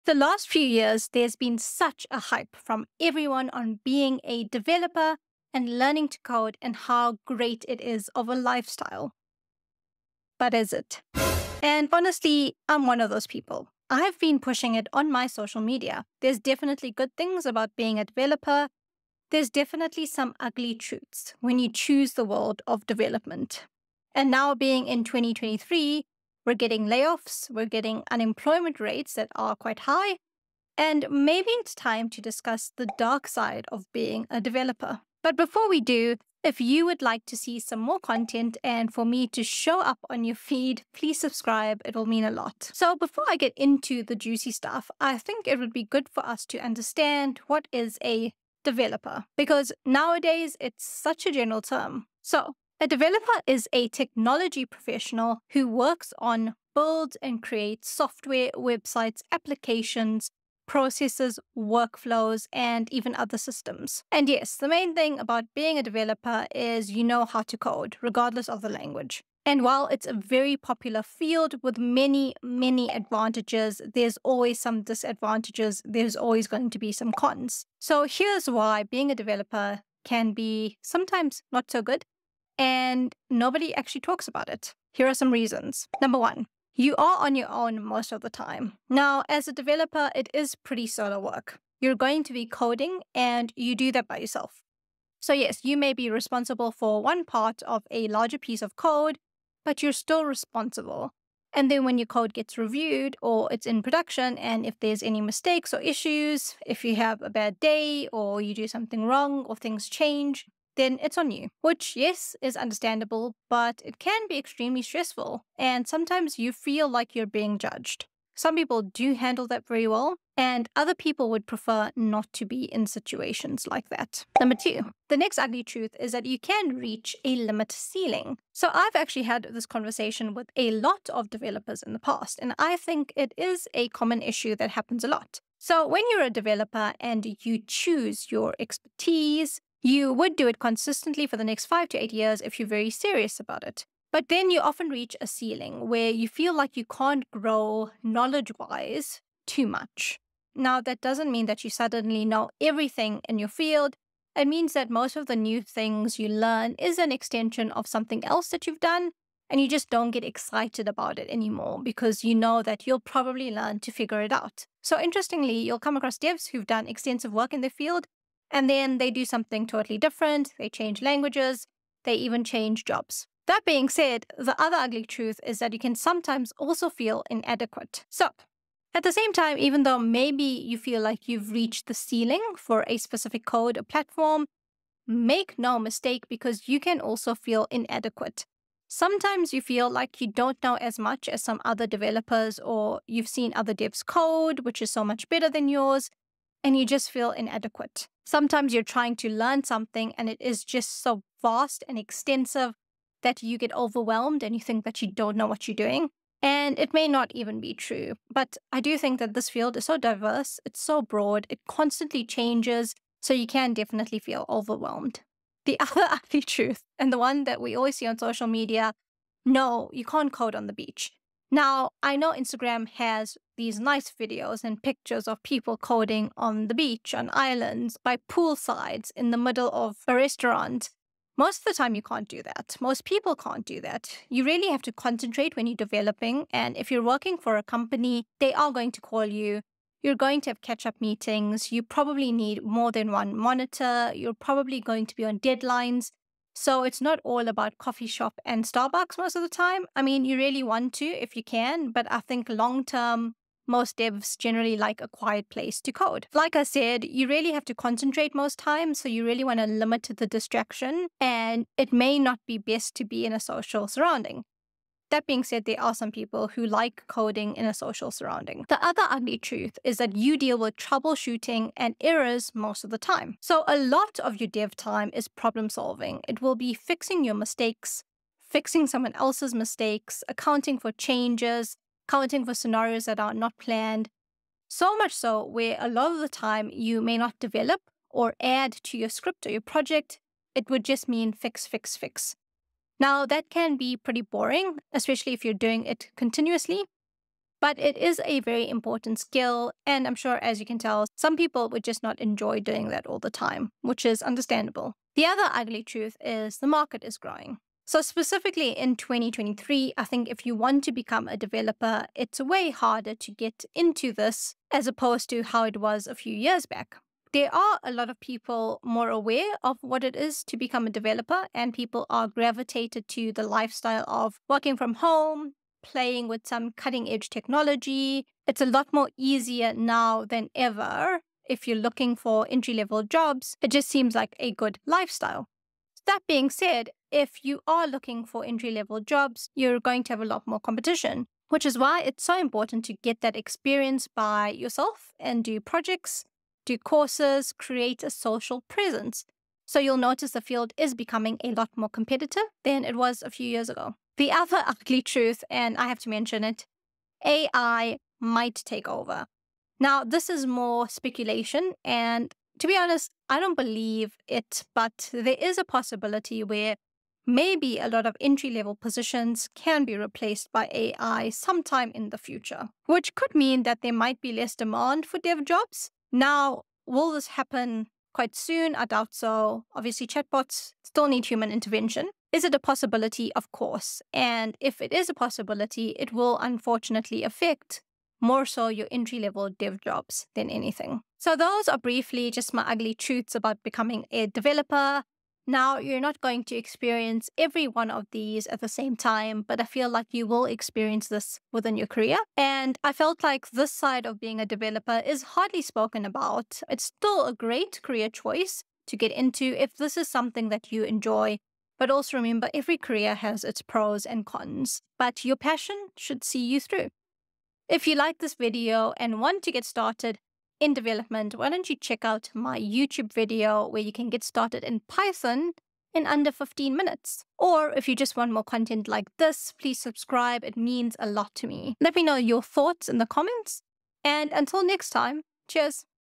What nobody tells you about Software Engineering
Everyone talks about how being a developer is a great career choice, but what about the not-so-great aspects? In this video, I'm sharing the ugly truths about being a developer that nobody talks about. From working alone most of the time to hitting a knowledge ceiling, feeling inadequate. Watch this video to learn more about what it's really like to be a developer!